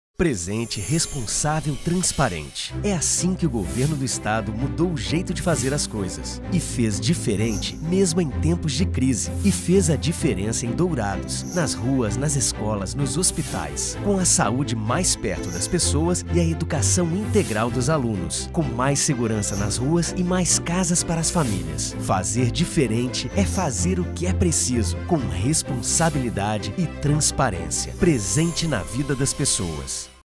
ENTREGAS REGIONAIS - DOURADOS
Para fazer diferente em tempos de crise e atender as prioridades dos sul-mato-grossenses, é preciso ser responsável com os recursos públicos. Tem sido assim em Dourados, com avanços na saúde, na segurança pública e na educação.